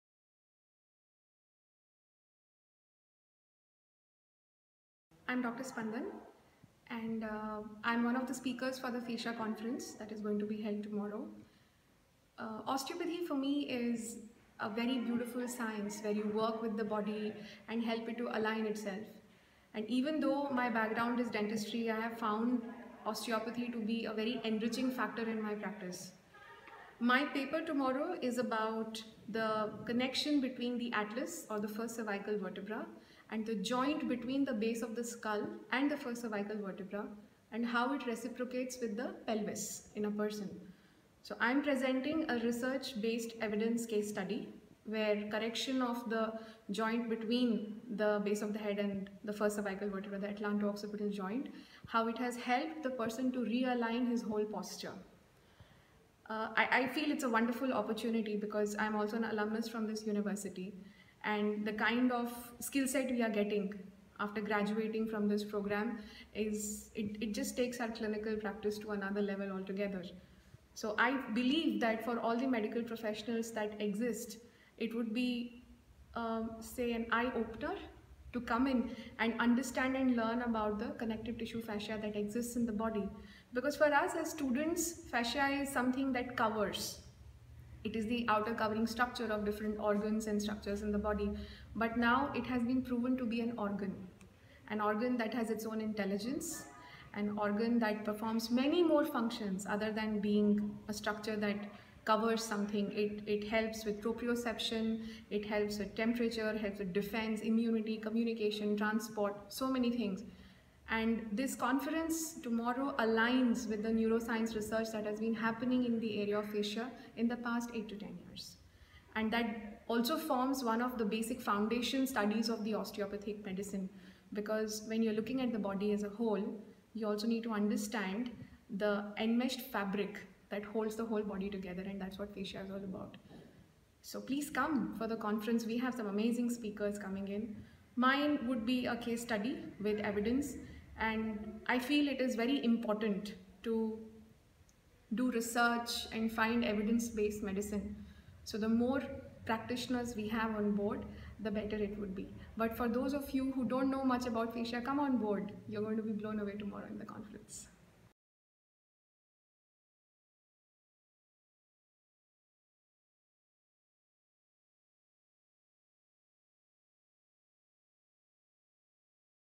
Osteopath - Dr. Spandan Katti
A dentist turned towards Cranial osteopathy inspired by her major recovery from a severe slip disc. 12 years of experience in Cranio-sacral work, with a post graduation degree in Osteopathy, from Sri Sri University. She is currently teaching with Body intelligence training, Sri Sri school of Yoga and has a home-based practice in Bengaluru.